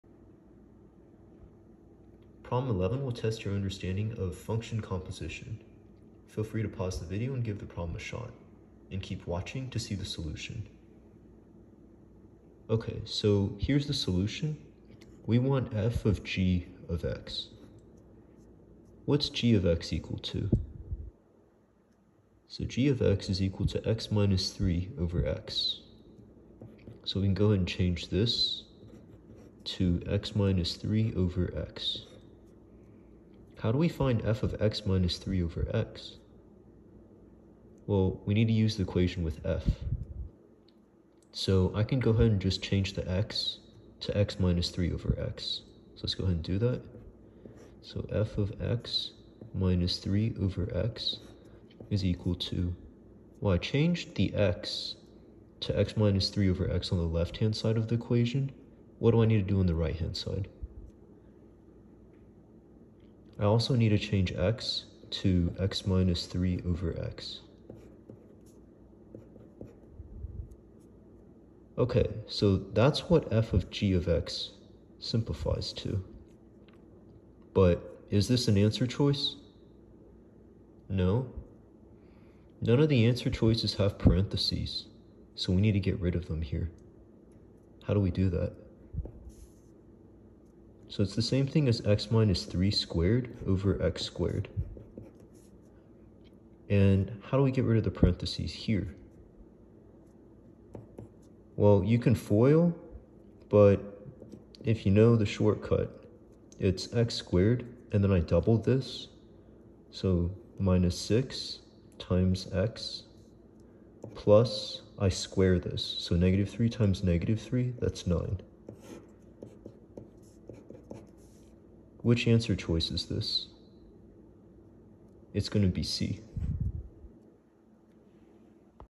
AP Precalculus Practice Exam - Composition of Functions
How to Solve Online AP Precalculus Practice Exam Multiple-Choice Part A Problem 11